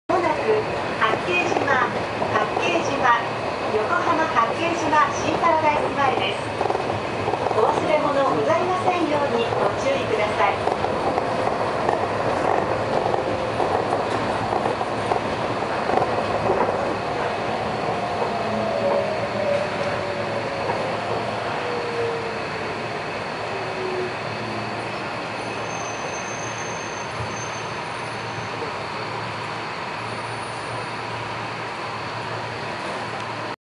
Riding the Kanazawa Seaside Line
The driverless train pulls into Hakkeijima station